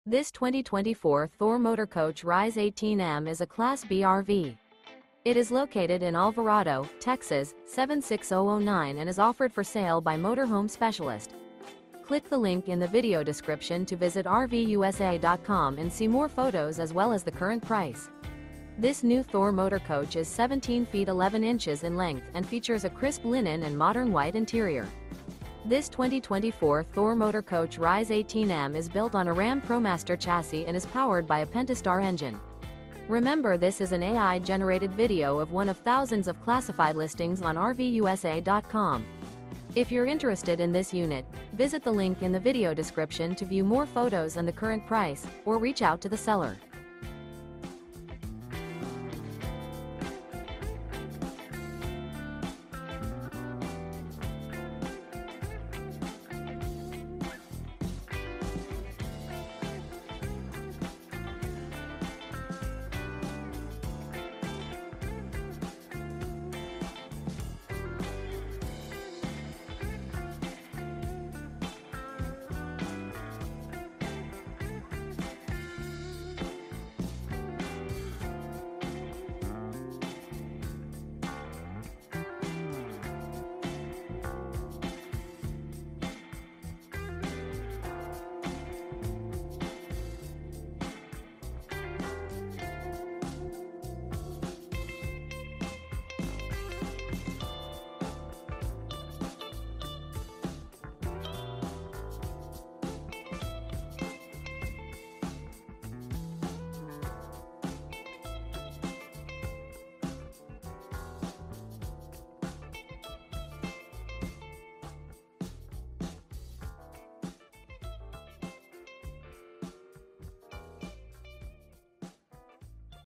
Magnificent 2024 Thor Motor Coach Rize Class B RV For Sale in Alvarado, TX | RVUSA.com
This 2024 Thor Motor Coach Rize 18M is a Class B RV. It is located in Alvarado, TX 76009 and is offered for sale by Motor Home Specialist. This New Thor Motor Coach Is 17' 11" in length and features a Crisp Linen & Modern White interior. This 2024 Thor Motor Coach Rize 18M is built on a RAM Promaster chassis and is powered by a Pentastar engine.